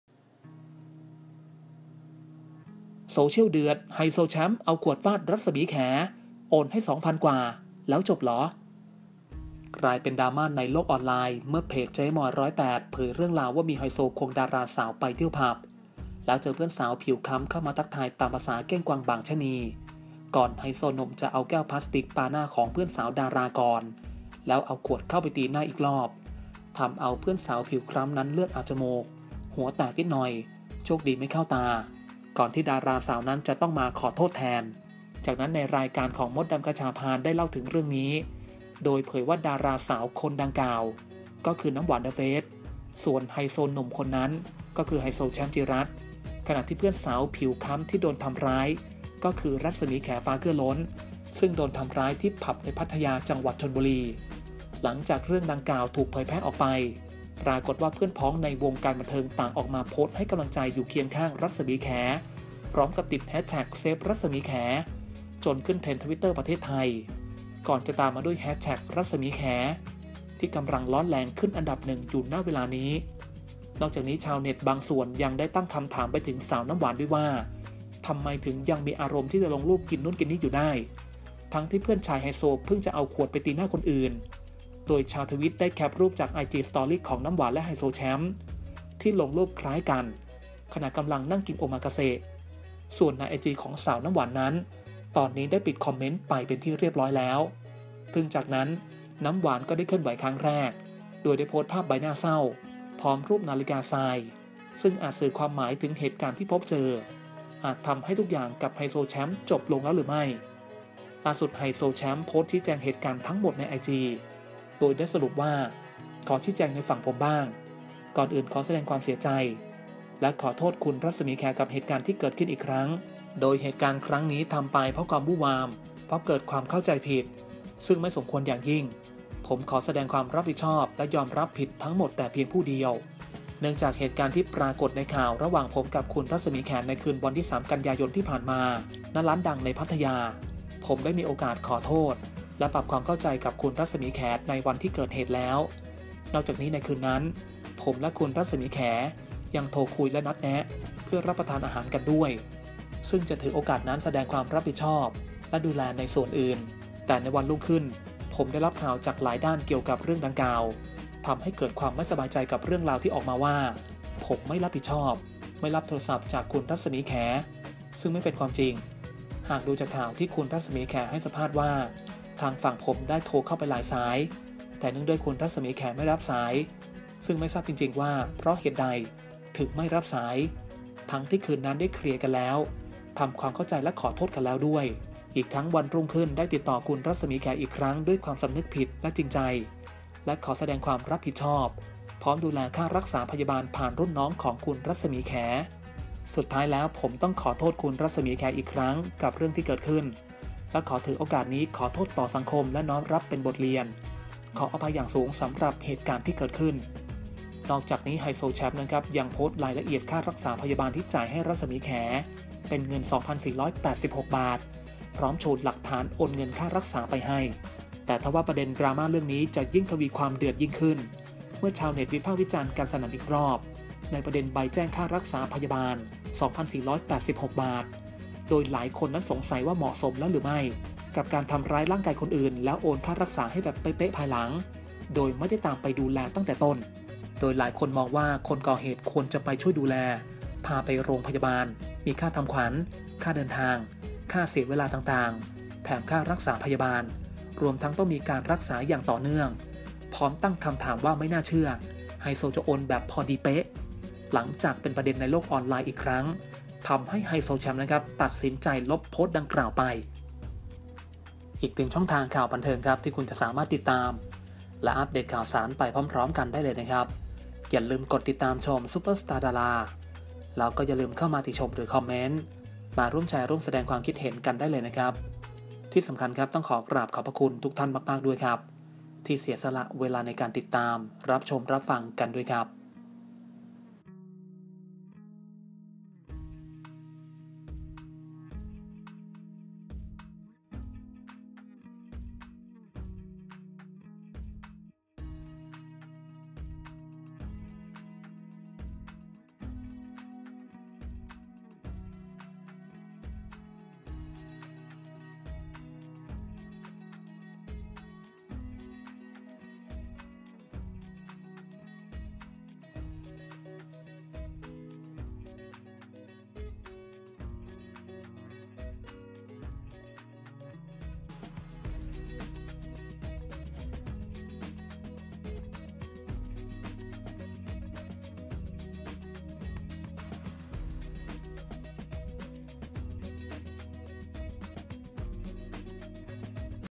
โซเชียลเดือด ไฮโซแชมป์ เอาขวดฟาด‘รัศมีแข’ โอนให้ 2 พันกว่าแล้วจบเหรอ#super star dara
โซเชียลเดือด ไฮโซแชมป์ เอาขวดฟาด‘รัศมีแข’ โอนให้ 2 พันกว่าแล้วจบเหรอ
เรื่องดังกล่าวถูกเผยแพร่ออกไป ปรากฏว่าเพื่อนพ้องในวงการบันเทิงต่างออกมาโพสต์ให้กำลังใจและอยู่เคียงข้าง รัศมีแข พร้อมติดแฮชแท็ก saveรัศมีแข จนขึ้นเทรนด์ทวิตเตอร์ประเทศไทย